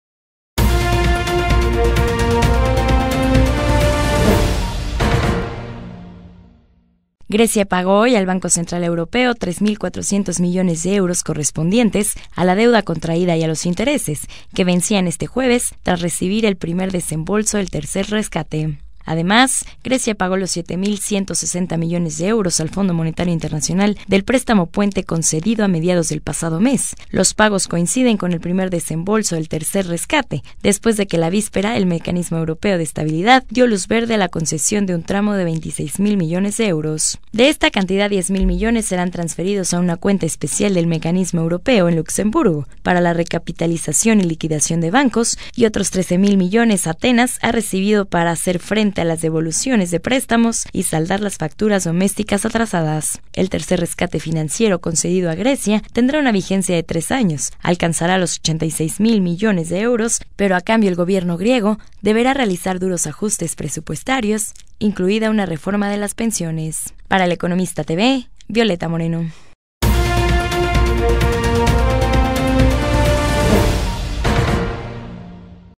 Grecia paga al BCE tres mil 400 millones de euros de su deuda
Grecia pagó hoy al Banco Central Europeo tres mil 400 millones de euros correspondientes a la deuda contraída y a los intereses, que vencían este jueves.

Otros tres mil millones de euros se desembolsarán “a más tardar el 30 de noviembre”, según el MEDE, en uno o dos tramos, sujetos a la implementación por parte de Grecia de medidas claves.

El tercer rescate financiero concedido a Grecia tendrá una vigencia de tres años y alcanzará los 86 mil millones de euros pero a cambio el gobierno griego deberá realizar duros ajustes presupuestarios, incluida una reforma de las pensiones.

Para vigilar que las reformas se implanten, habrá evaluaciones cada tres meses, la primera de ellas el próximo octubre.